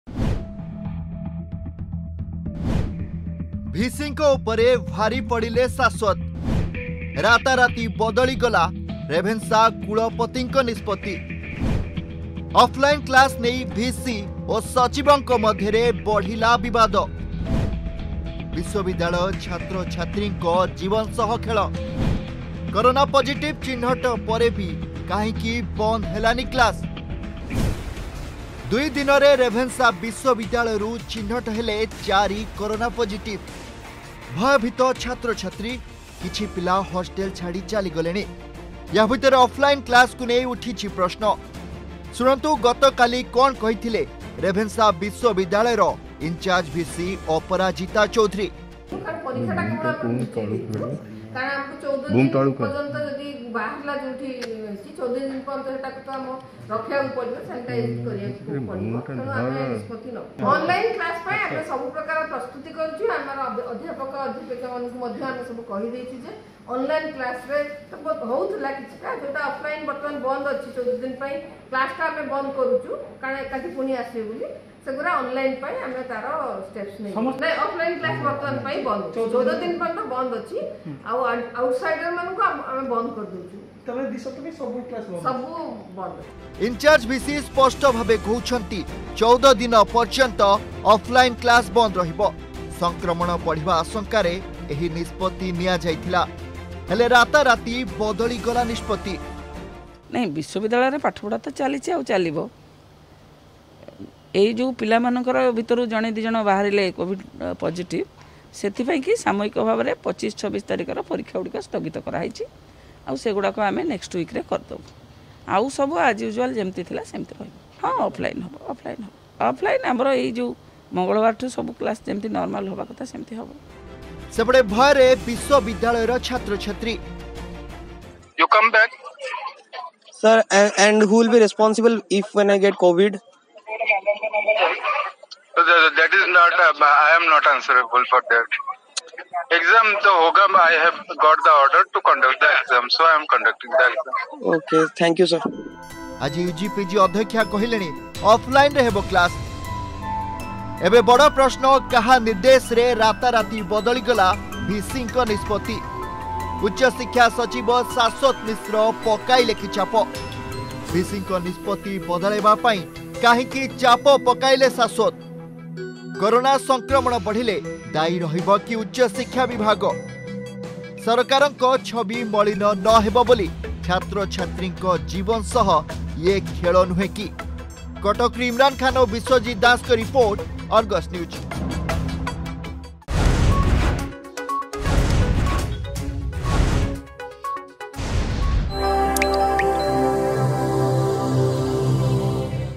ଭିସିଙ୍କ ଉପରେ ଭାରି ପଡିଲେ ଶାଶ୍ବତ ...
ରାତାରତି ବଦଳିଗଲା ନିଷ୍ପତ୍ତି । 4 କରୋନା ପଜିଟିଭ୍ ଚିହ୍ନଟ ହେବା ପରେ ବି ରେଭେନ୍‌ସା ବିଶ୍ବବିଦ୍ୟାଳୟରେ କ୍ଲାସ ବନ୍ଦ ହେବନାହିଁ । ଯାହାକୁ ନେଇ ଛାତ୍ରଛାତ୍ରୀ , ଅଧ୍ୟାପପକ ଅଧ୍ୟାପିକା ଓ କର୍ମଚାରୀଙ୍କ ମଧ୍ୟରେ ଅସନ୍ତୋଷ ବଢିବାରେ ଲାଗିଛି ...